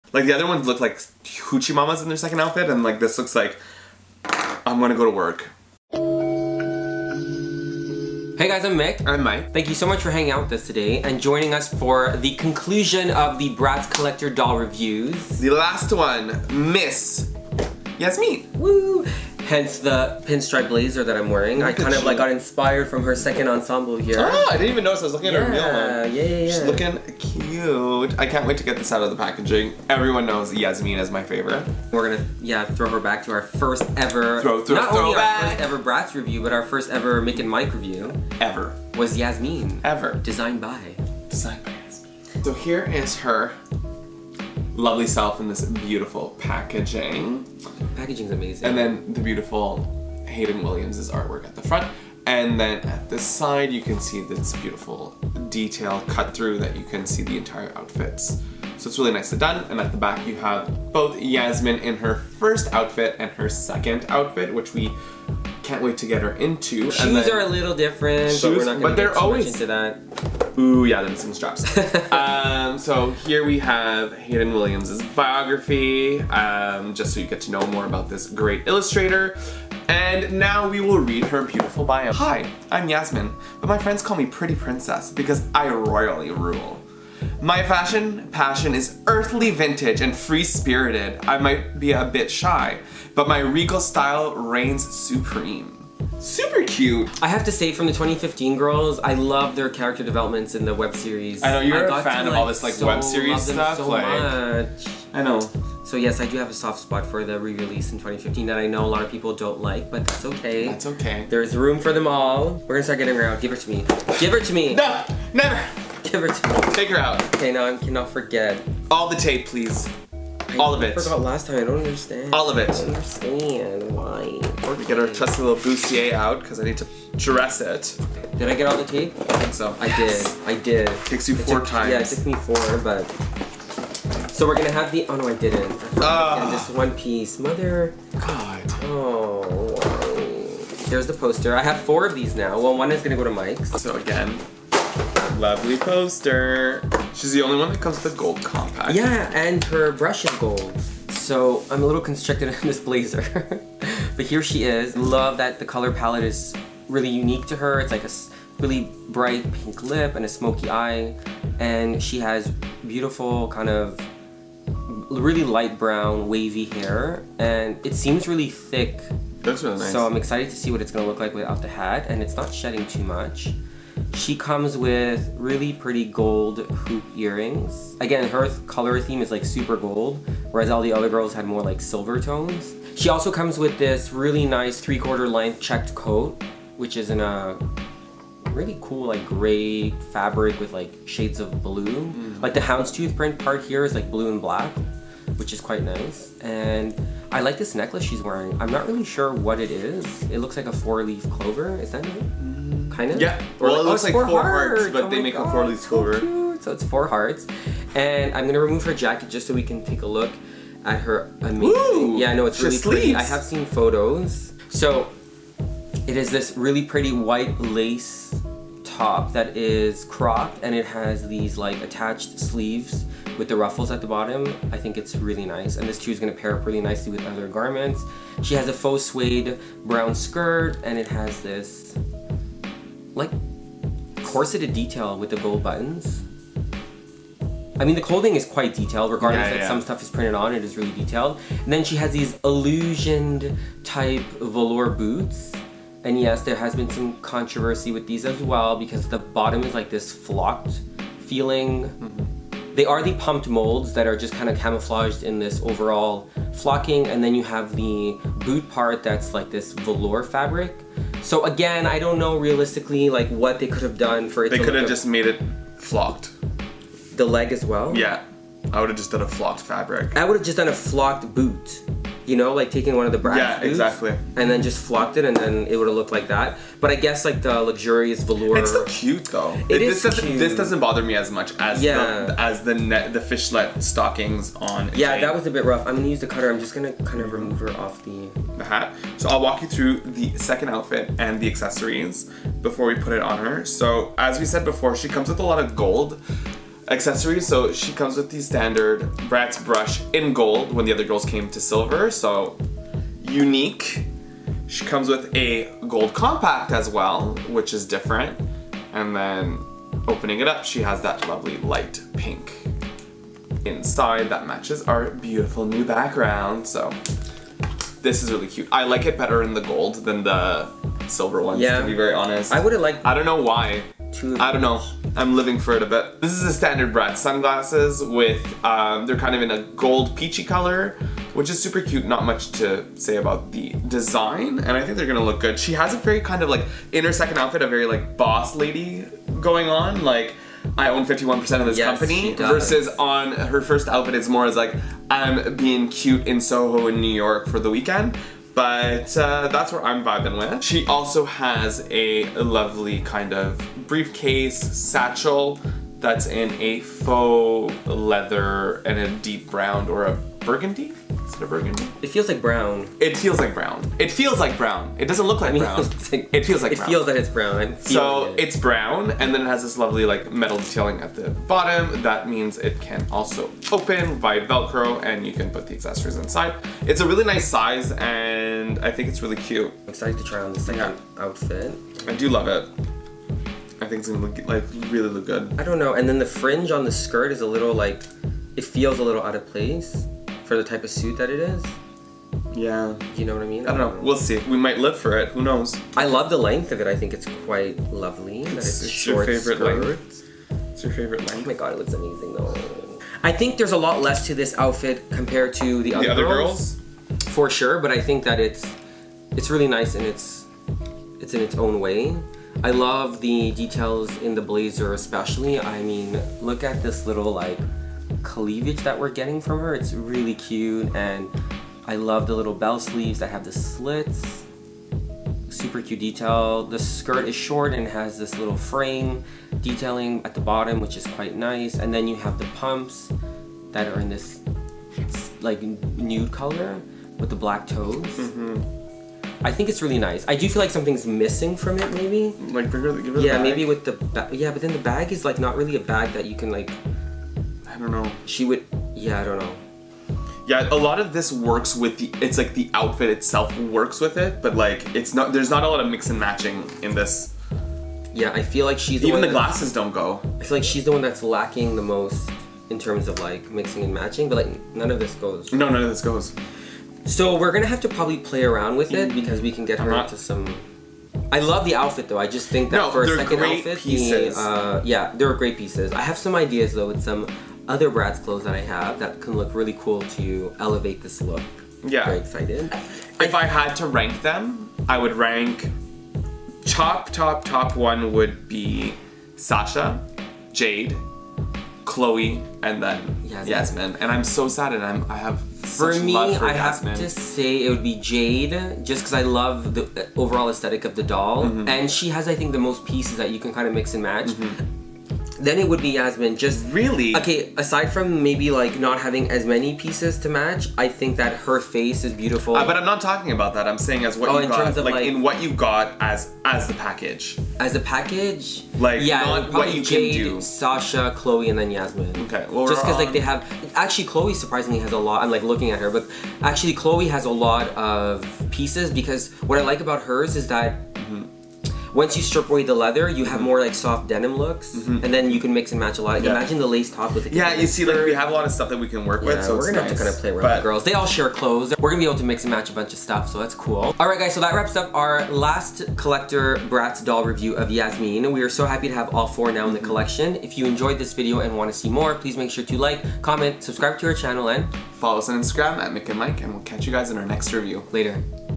Bratz Collector: Yasmin - Doll Unboxing and Review
Hey guys!

She's finally here! Join us as Yasmin is reunited with her besties and let us know which of the girls are your favourites!

We can't believe that our first video is already two years old! Take a stroll down memory lane with us and watch our first ever Yasmin review 🥰.

Music_